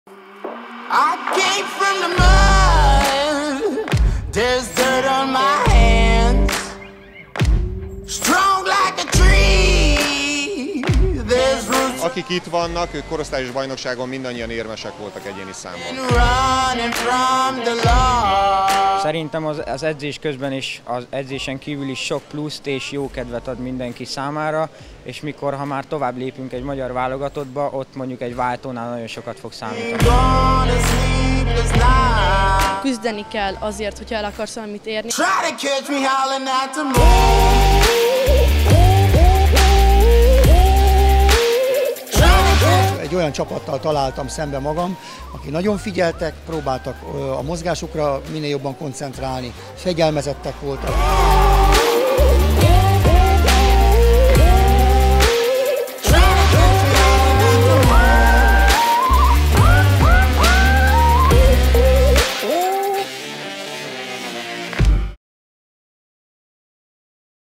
Jövő Bajnokai a technikai edzőtáborról
A százhalombattai Jövő Bajnokai edzőtábor különleges volt, hiszen elsősorban a technika javítására helyezték a hangsúlyt.